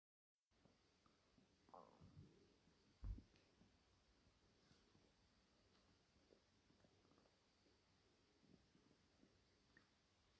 All Of My Entire Cats
I Got 4 Cats At Mams, And 2 Cats At Dads

The Cats At Mams:
1. Calon
2. Celyn
3. Cariad
4. Candy

The Cats At Dads:
1. Reuben
2. Fluffy